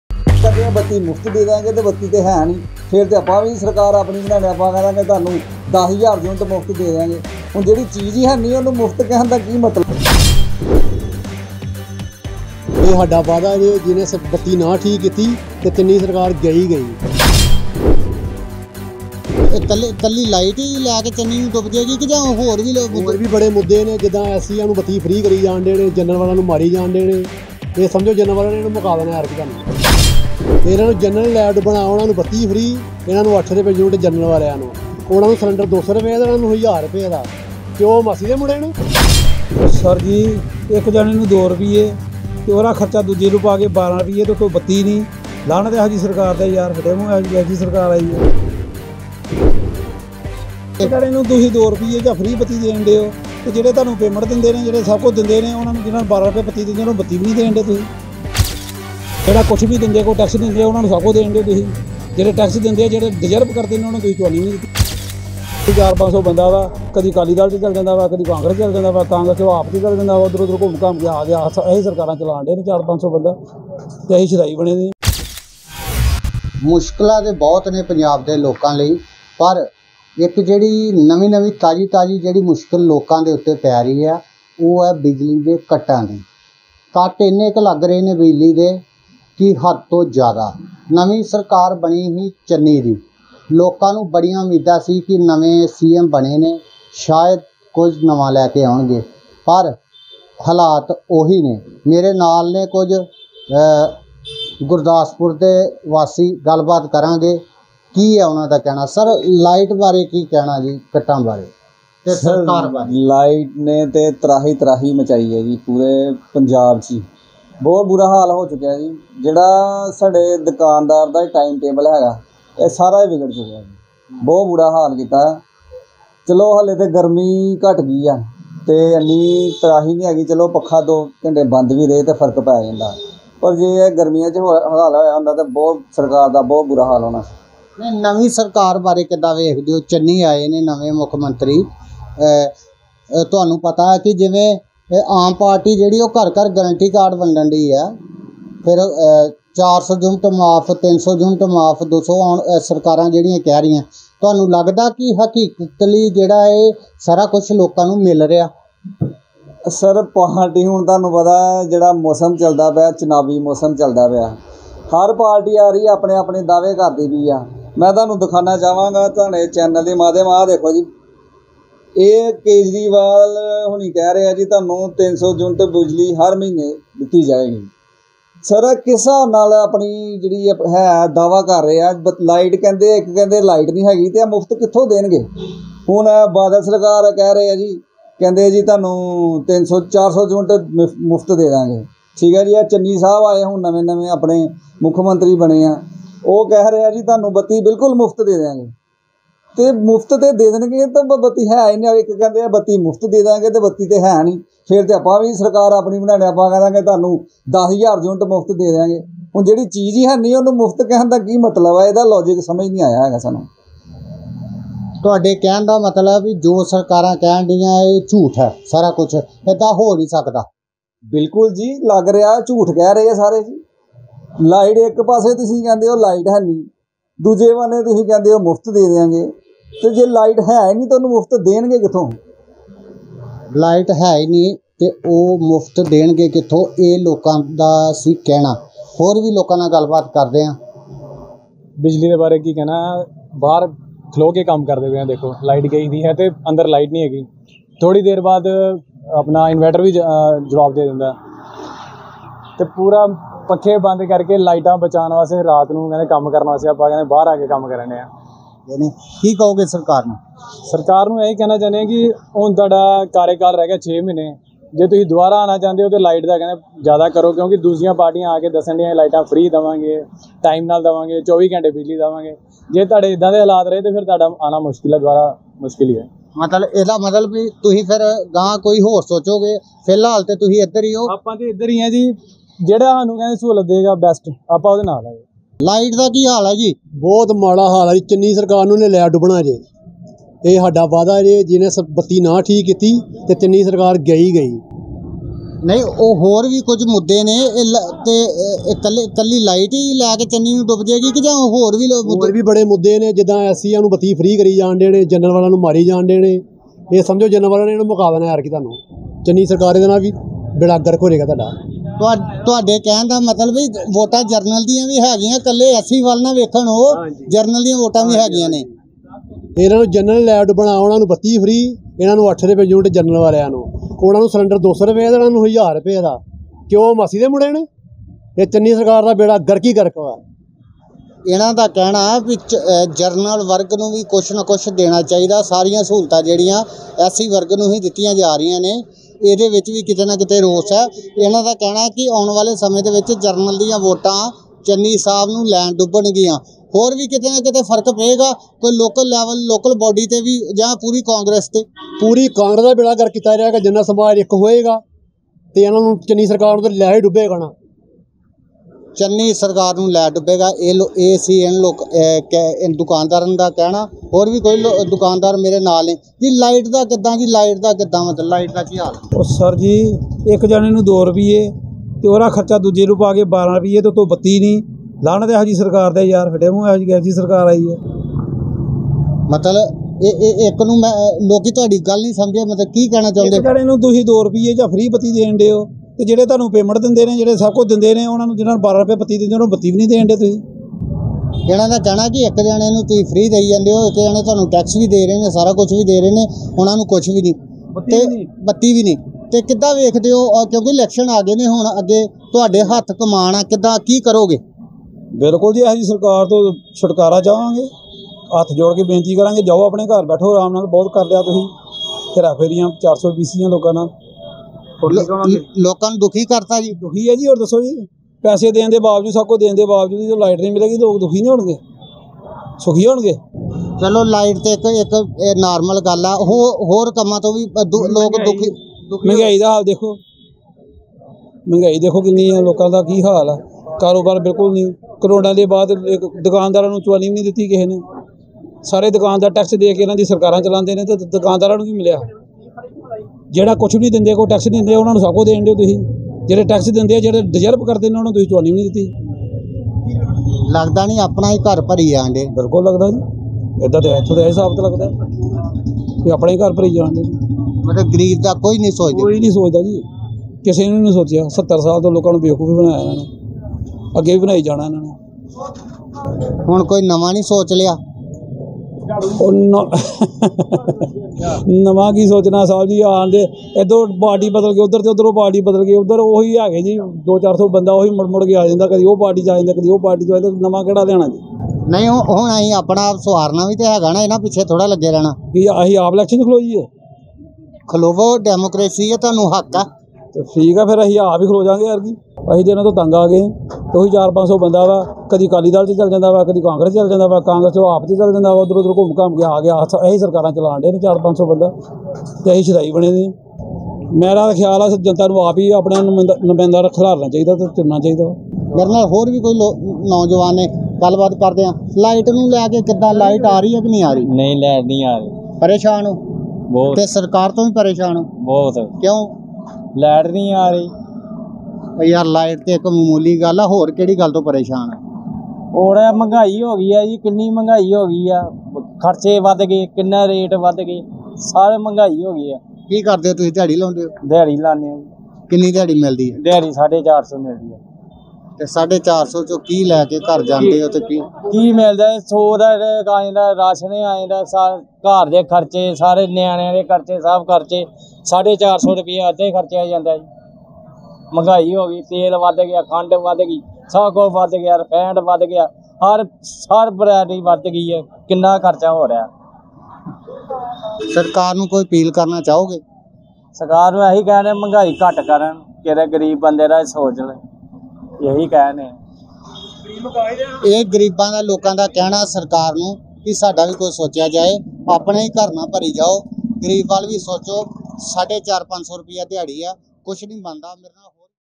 ਬਿਜਲੀ ਦੀ ਘਾਟ ਨੂੰ ਲੈਕੇ ਗੁਰਦਾਸਪੁਰੀਆਂ ਨੇ ਚੰਨੀ ਸਰਕਾਰ ਨੂੰ ਪਾਈਆਂ ਲਾਹਨਤਾਂ, ਜਰਨਲ ਵਾਲਿਆਂ ਨੂੰ ਸ਼ਦਾਈ ਬਣਾਇਆ
| Tez Punjab News |